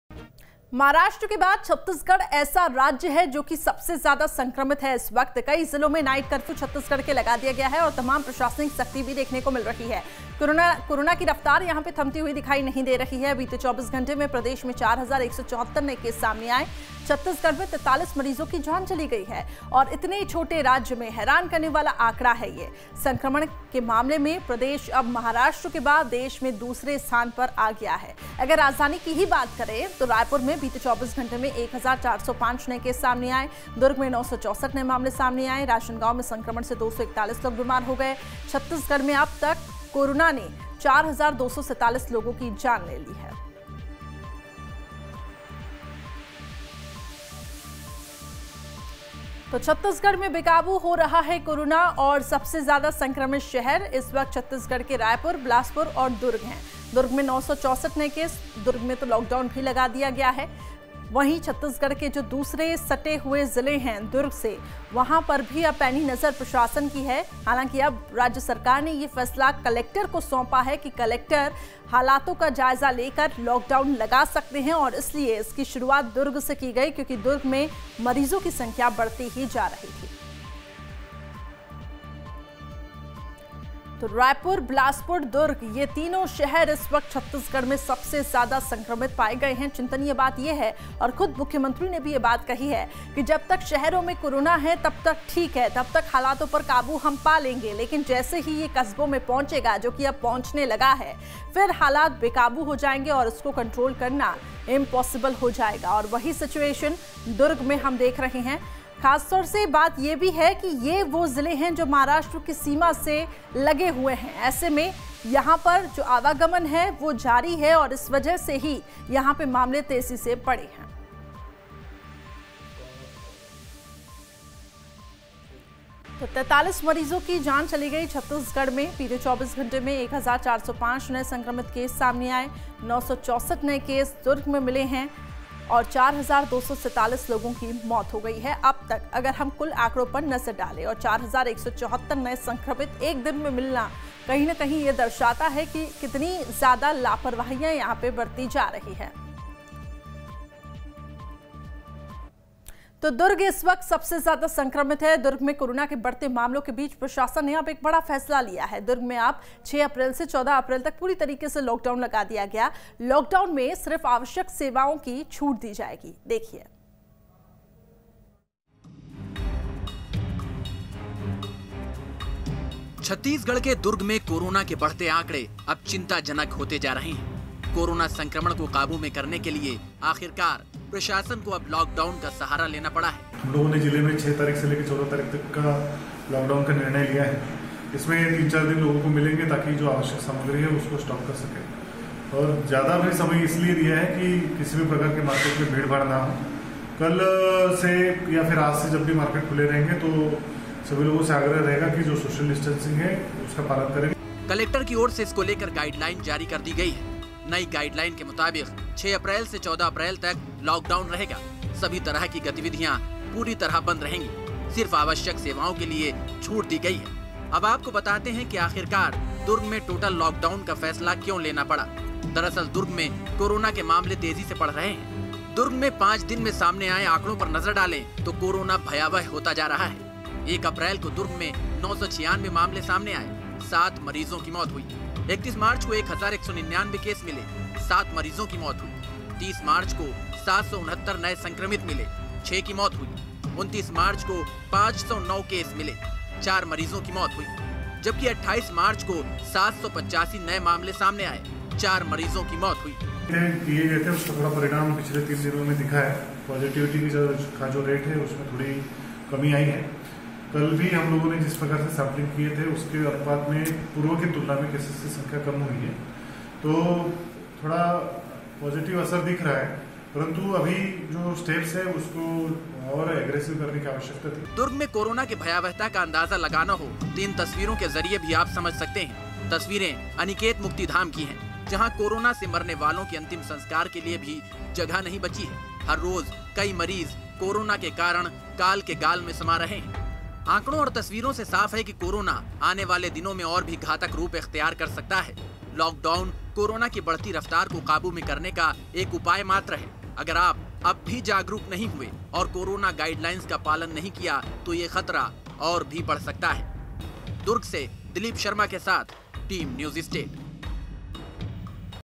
Chhattisgarh: CG में लगातार बढ़ती जा रही है कोरोना मरीजों की संख्या, देखें रिपोर्ट
News State Apps डाउनलोड करें:
Download Android and iOS Apps for more updates: